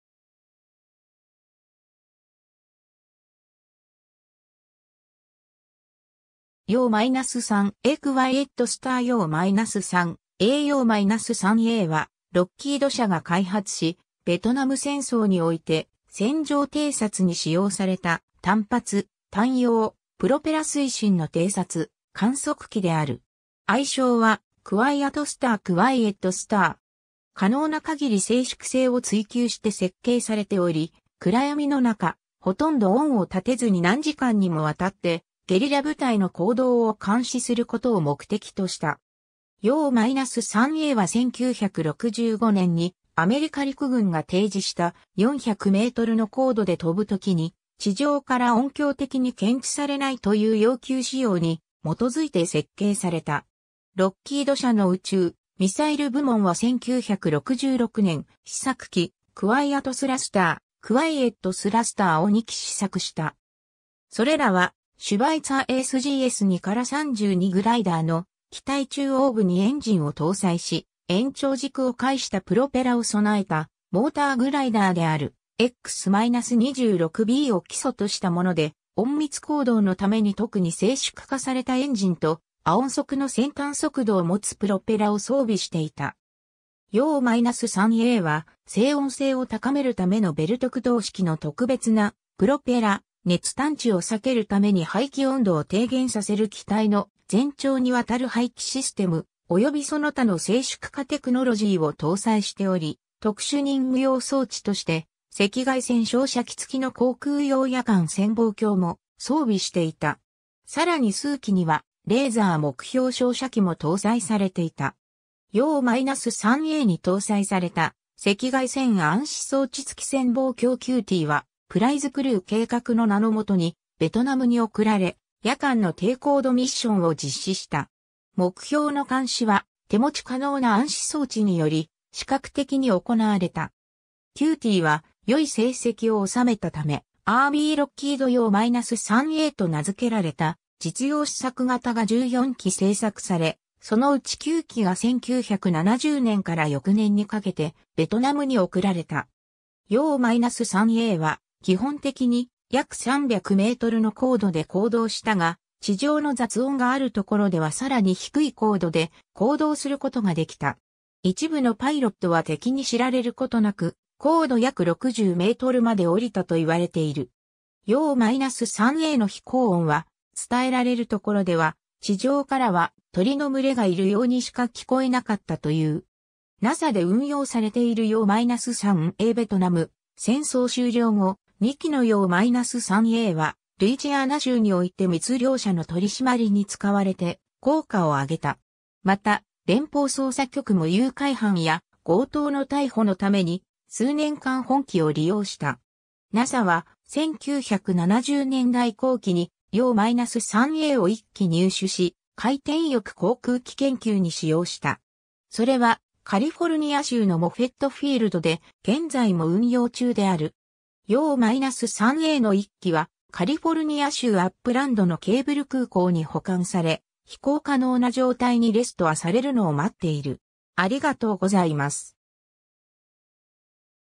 YO-3A (航空機)
YO-3A Quiet Star YO-3A YO-3Aはロッキード社が開発し、ベトナム戦争において戦場偵察に使用された単発・単葉・プロペラ推進の偵察・観測機である。
愛称は「クワイアト・スター "Quiet Star"（“静かな星”の意）」。
 可能な限り静粛性を追求して設計されており、暗闇の中、ほとんど音を立てずに何時間にもわたってゲリラ部隊の行動を監視することを目的とした。
 YO-3Aは1965年にアメリカ陸軍が提示した「400 m（1,200 ft）の高度で飛ぶときに地上から音響的に検知されない」という要求仕様に基づいて設計された。
 ロッキード社の宇宙・ミサイル部門は1966年、試作機「クワイアト・スラスター、"Quiet Thruster"（“静かに進むもの”の意）」を2機（QT-1、QT-2）試作した。
それらはシュバイツァーSGS2-32グライダーの機体中央部にエンジンを搭載し、延長軸を介したプロペラを備えたモーターグライダーであるX-26Bを基礎としたもので、隠密行動のために特に静粛化されたエンジンと、亜音速の先端速度を持つプロペラを装備していた。
 YO-3Aは、静音性を高めるためのベルト駆動式の特別なプロペラ、熱探知を避けるために排気温度を低減させる機体の全長にわたる排気システム、およびその他の静粛化テクノロジーを搭載しており、特殊任務用装置として赤外線照射器付きの航空用夜間潜望鏡も装備していた。
さらに数機には、レーザー目標照射機も搭載されていた。
 YO-3Aに搭載された赤外線暗視装置付き潜望鏡 QTは「プライズ・クルー（Prize Crew）」計画の名のもとにベトナムに送られ、夜間の低高度ミッションを実施した。
目標の監視は、手持ち可能な暗視装置により視覚的に行われた。
QTは良い成績を収めたため、アーミー・ロッキードYO-3Aと名づけられた実用試作型が14機（シリアル69-18000～18013）製作され、そのうち9機が1970年から翌年に掛けてベトナムに送られた。
 YO-3Aは基本的に約300 m（1,000 ft）の高度で行動したが、地上の雑音が有るところではさらに低い高度で行動することができた。
一部のパイロットは敵に知られることなく高度約60 m（200 ft）まで降りたと言われている。
YO-3Aの飛行音は、伝えられるところでは、地上からは「鳥の群れがいる」ようにしか聞こえなかったという。
 NASAで運用されているYO-3A （1997年の撮影） ベトナム戦争終了後、2機のYO-3A（69-18006、-18007）は、ルイジアナ州において密漁者の取り締まりに使われて効果を上げた。
また、連邦捜査局（FBI）も誘拐犯や強盗の逮捕のために、数年間本機を利用した。
 NASA（アメリカ航空宇宙局）は、1970年代後期にYO-3Aを1機入手し、回転翼航空機研究（rotorcraft research）に使用した。
それはカリフォルニア州のモフェットフィールドで、現在も運用中である。
 YO-3Aの1機（シリアル69-18007）はカリフォルニア州アップランドのケーブル空港に保管され、飛行可能な状態にレストアされるのを待っている。